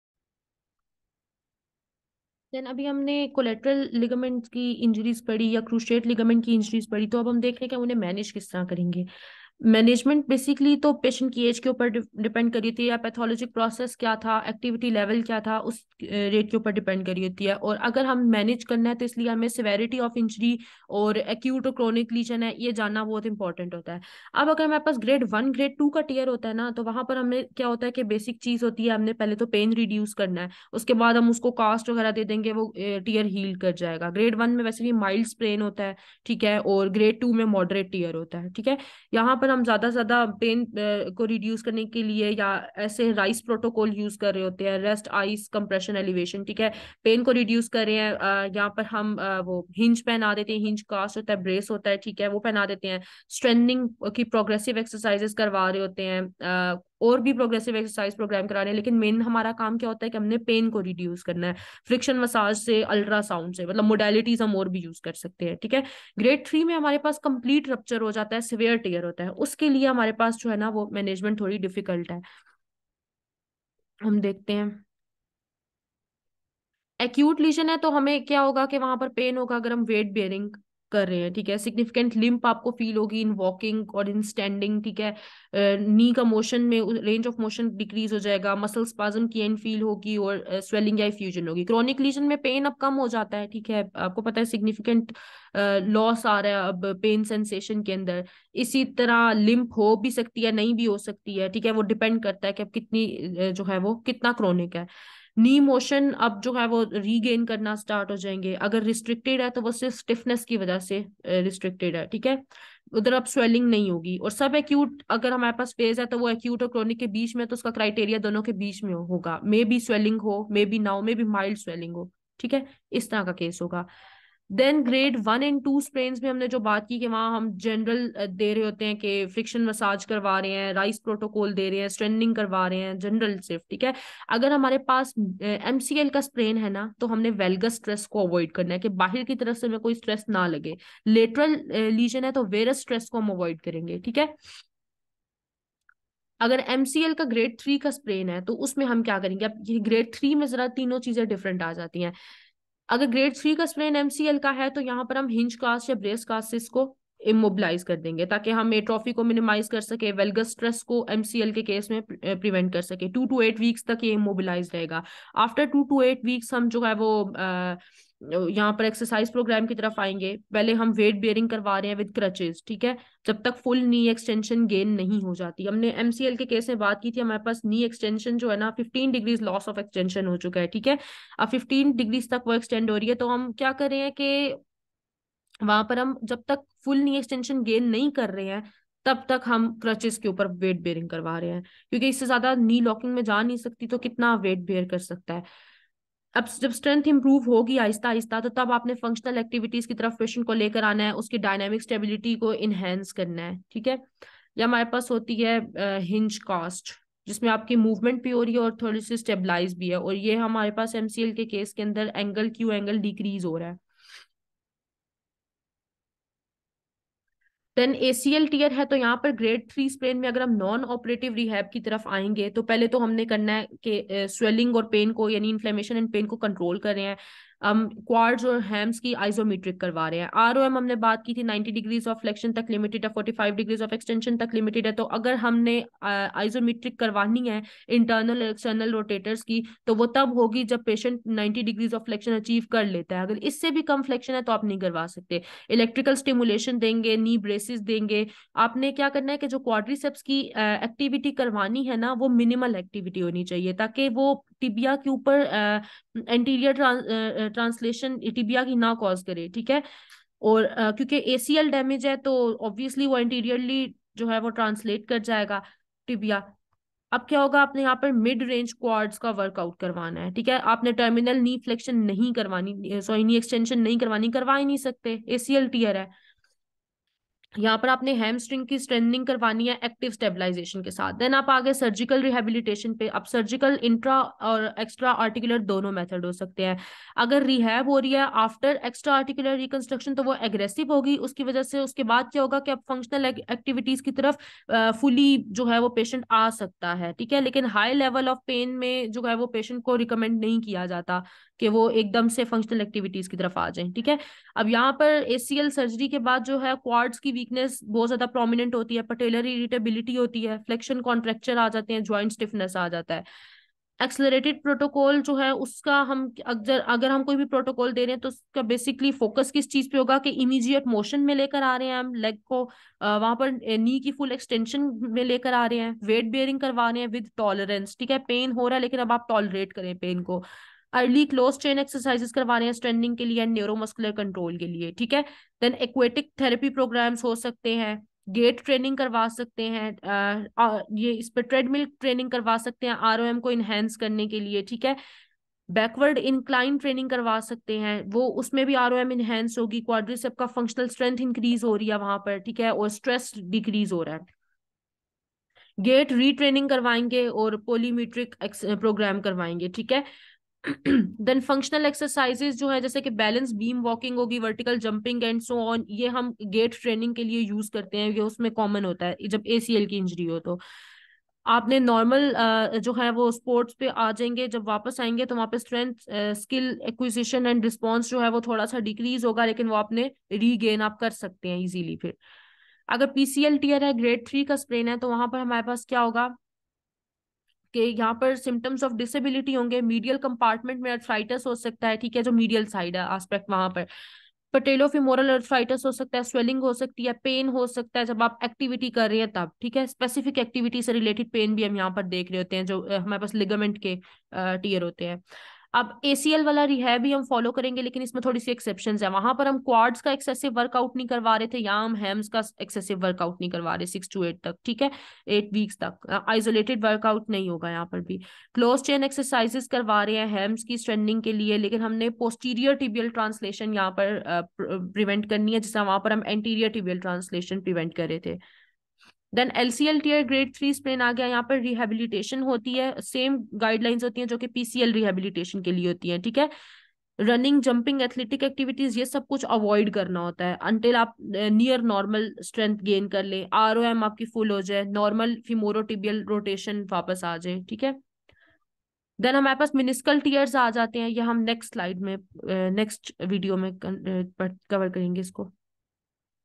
Knee Pathologies | Part 3 | Injuries
Common knee pathologies in musculoskeletal physiotherapy.